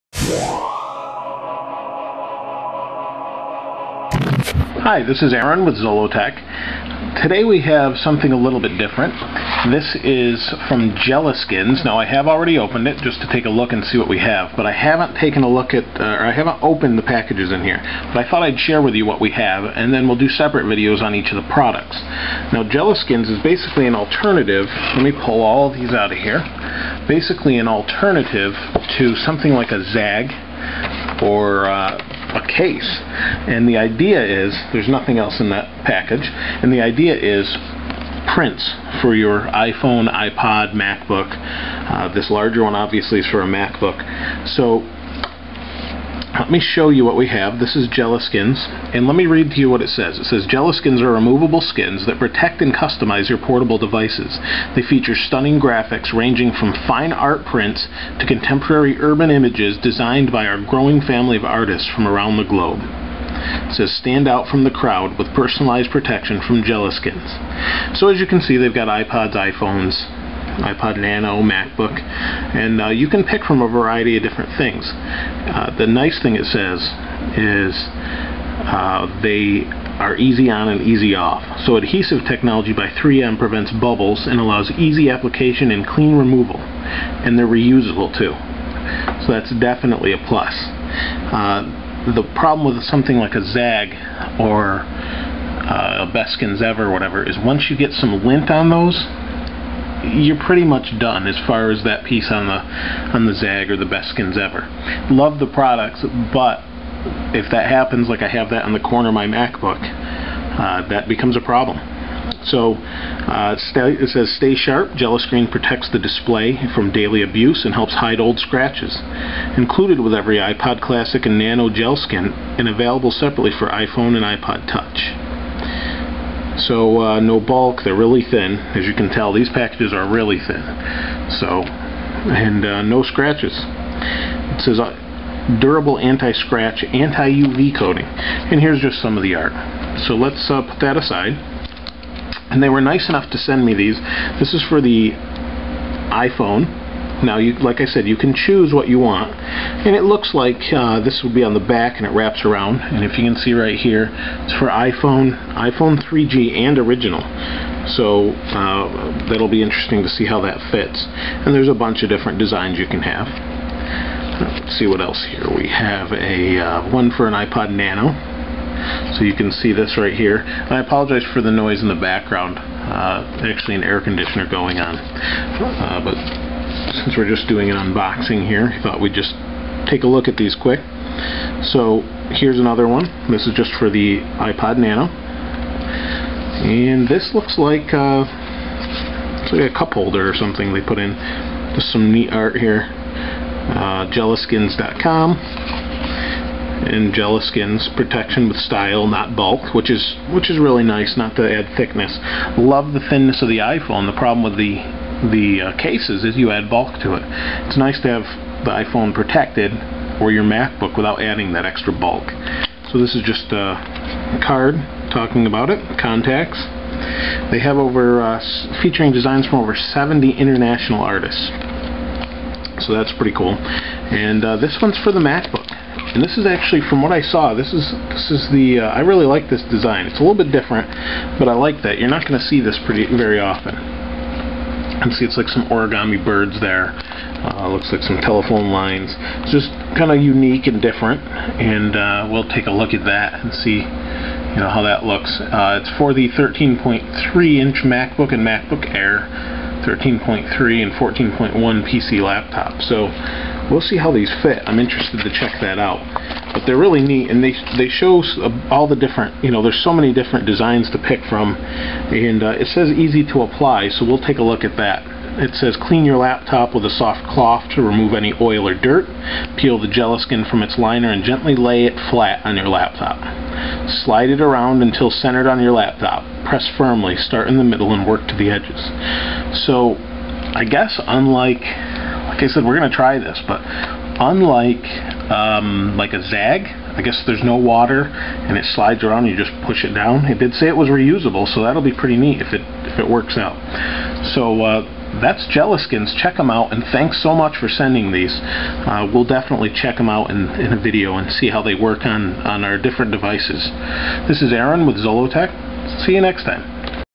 GelaSkins Unboxing - iPhone, iPod & MacBook
Today GelaSkins was nice enough to send along a couple of their covers for iPhone 3G/3GS, iPod Nano and MacBook.  We take a look at what they sent.